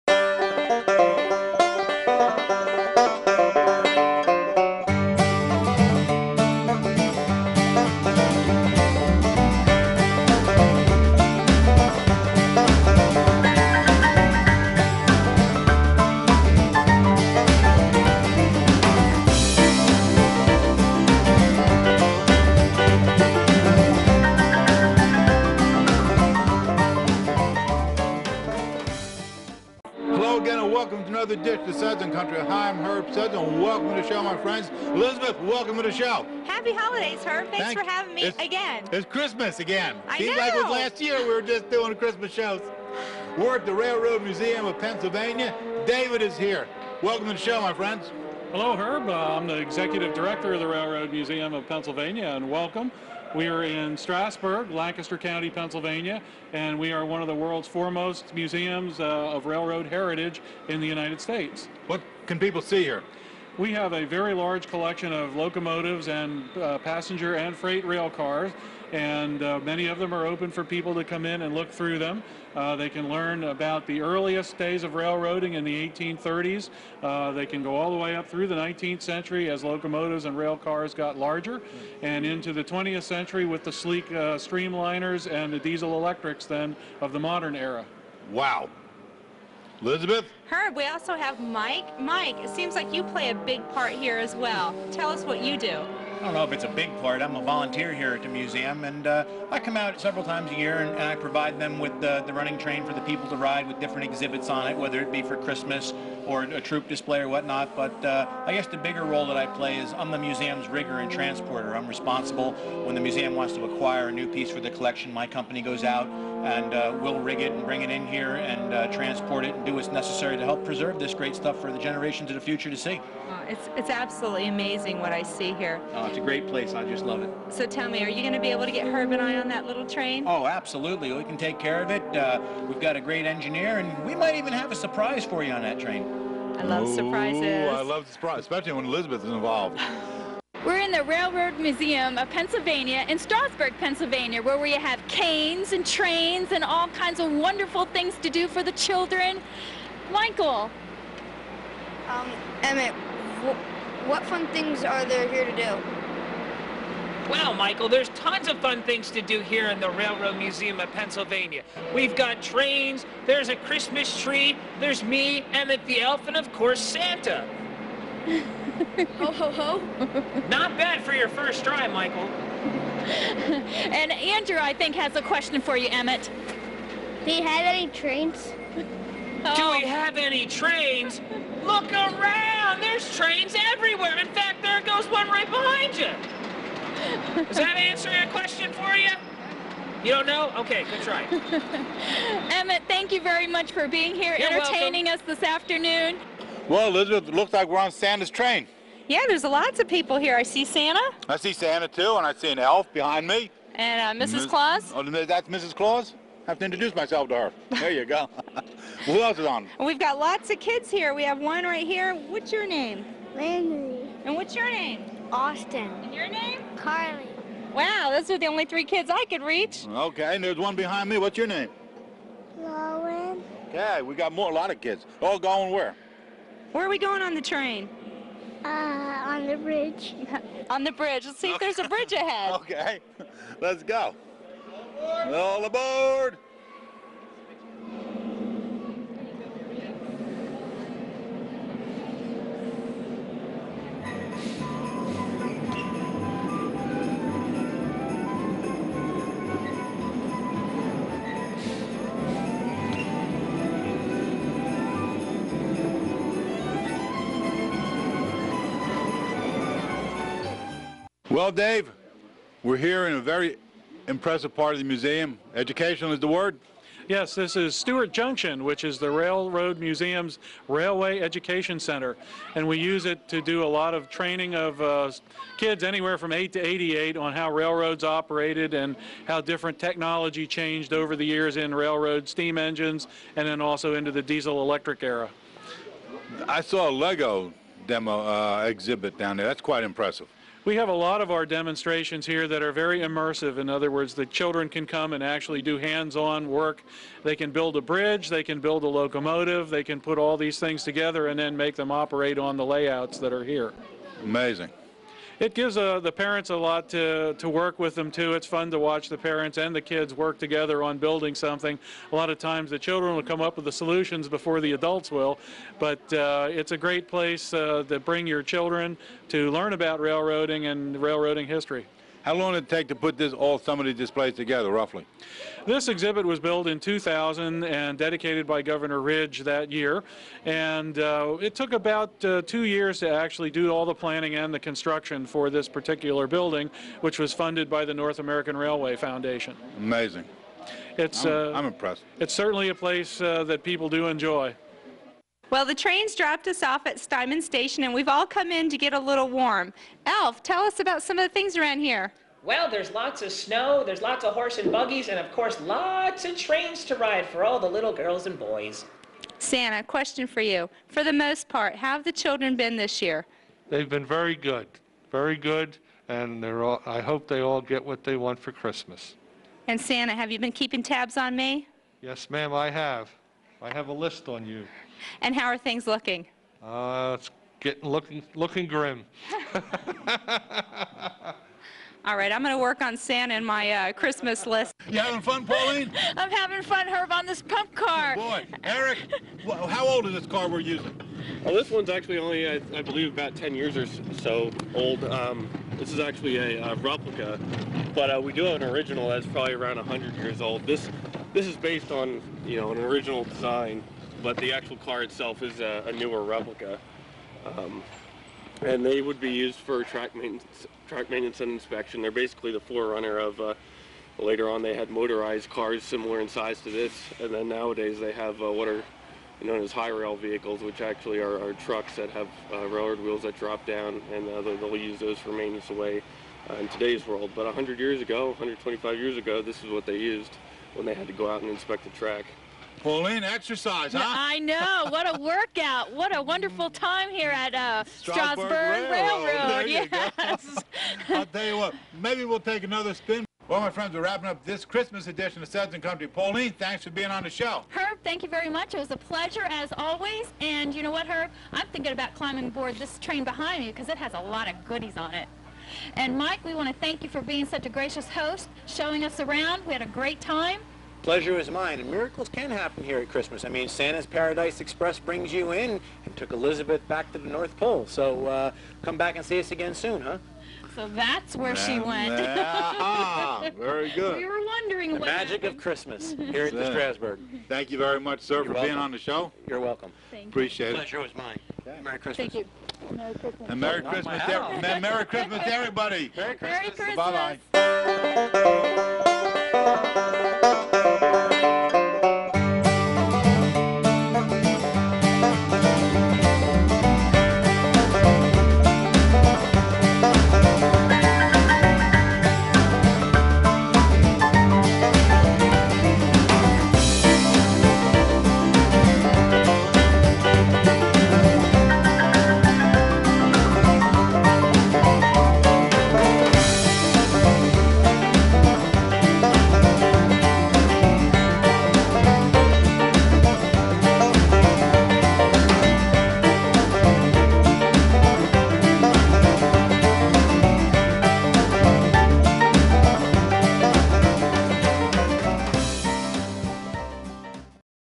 Christmas Time at the Railroad Museum of Pennsylvania
Pauline, Elizabeth and Herb check out Christmas on the rails. This museum is all railroad. From the 1830's through almost today. Locomotives, boxcars, passenger cars, freight cars and more all decorated for the Christmas season. Get into the holiday spirit railroad style.